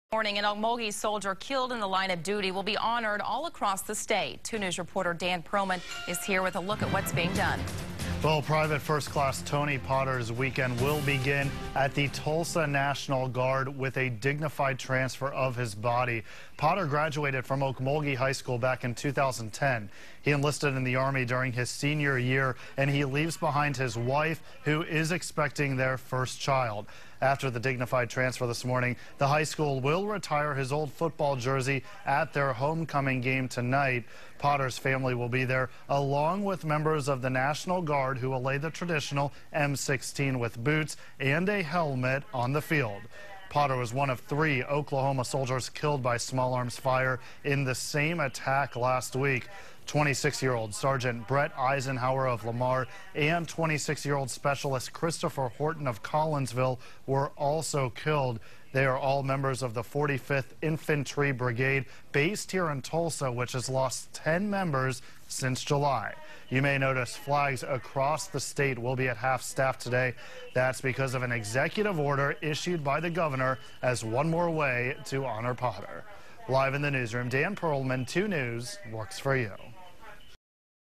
Okmulgee soldier honored across state
A fallen Oklahoma soldier is being honored this weekend.